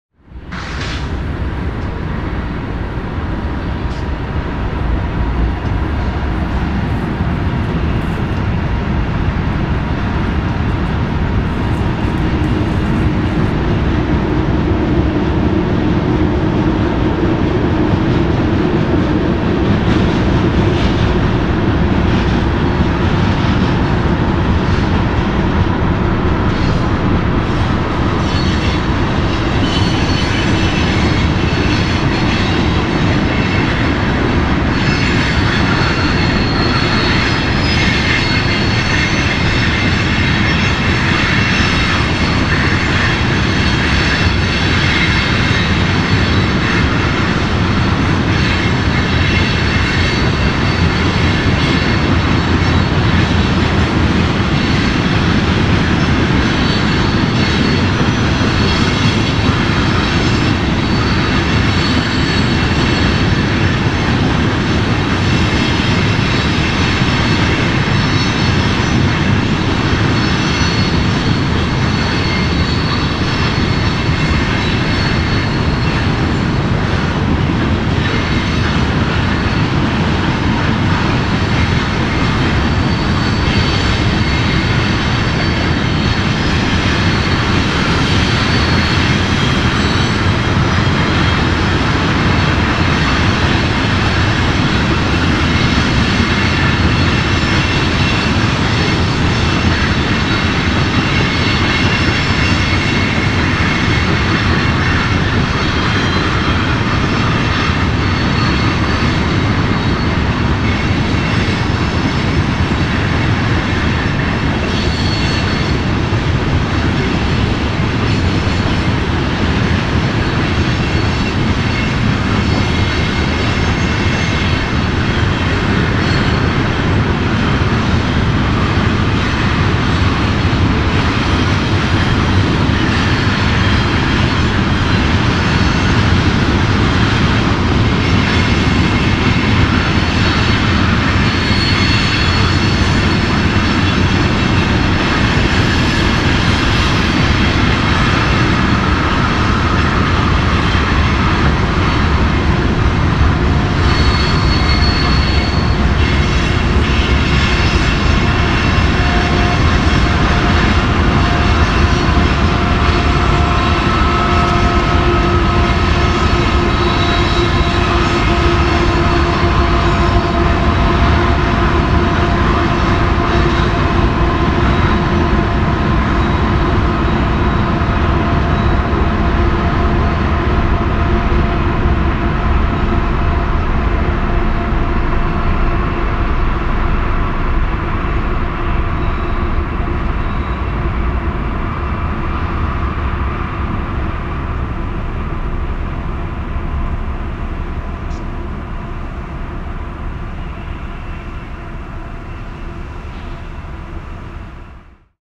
Drone Video: Eastbound BNSF grain train enters Shed 12
An Eastbound enters Shed 12, just east of Essex MT.